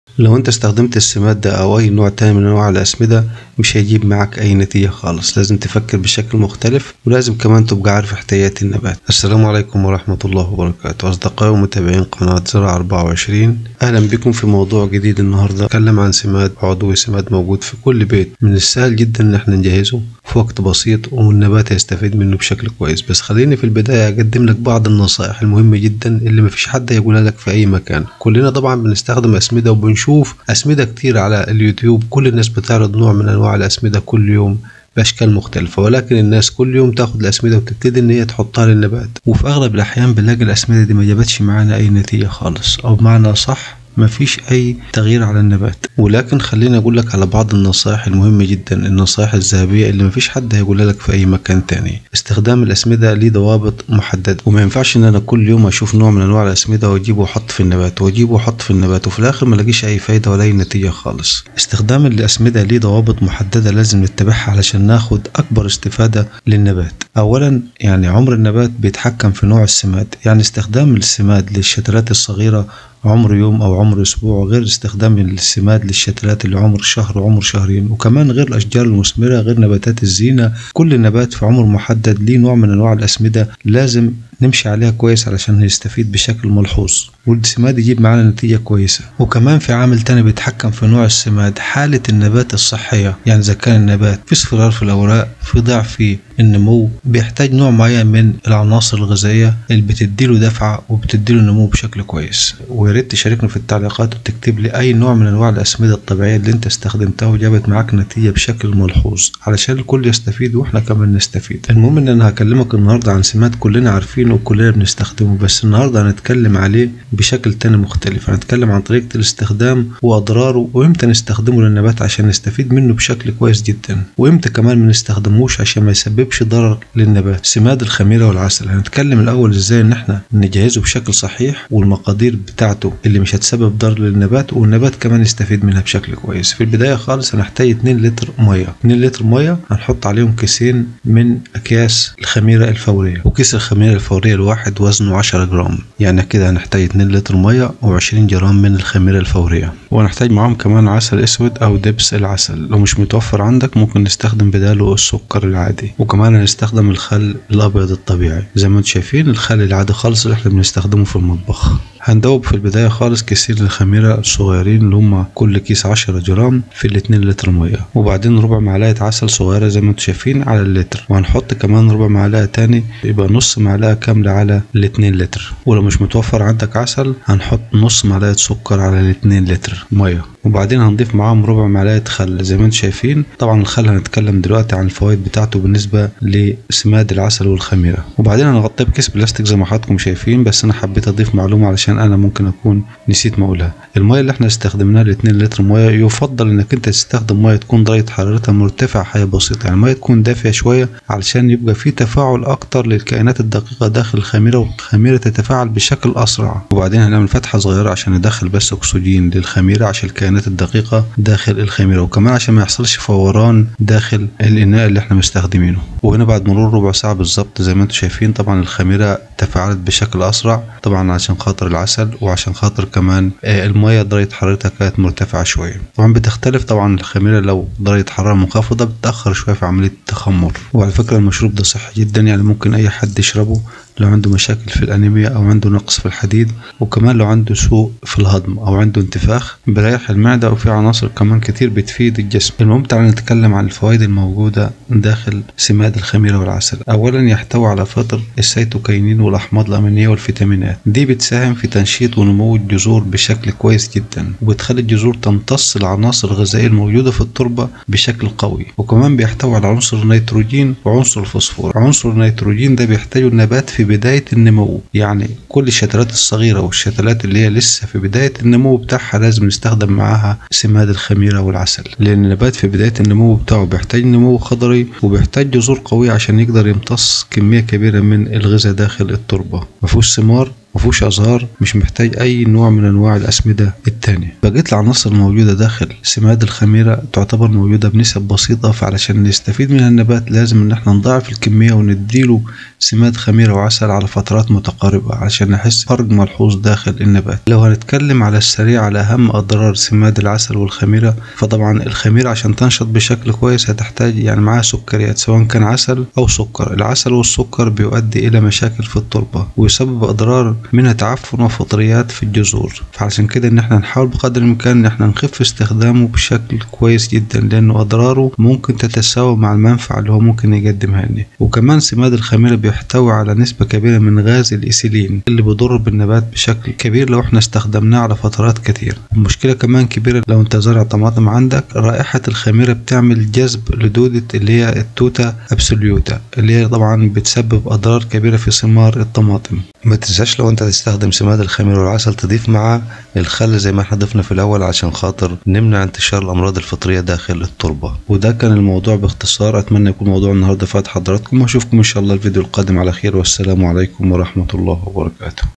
تنمو جميع الشتلات الصغيرة بجنون عند اضافة هذا السماد المتوفر فى بيتك
اقوى الاسمدة الطبيعية المتوفرة فى كل بيت سماد الخميرة والعسل متى يمكن استخدامة ومع اى نبات يعطى نتيجة فعالة ونتيجة مرضية
استخدام سماد الخميرة والعسل مع الشتلات الصغيرة يعطى نتيجة طيبة وملحوظة احسن وافضل مع اى نوع اخر من النباتات حيث يحتوى سماد الخميرة على عنصر النيتروجين الذى يعطى نمو خضرى وجذرى للنبات فى عمرة الصغير سماد طبيعي وفعالة
اهم اضرار استخدام سماد الخميرة والعسل يسبب عفن فى الجذور ورائحة الخميرة تعمل جذب للحشرات وخصوصا حشرة التوتا ابسيلوتا فى الطماط التى تسبب اضرار بالغة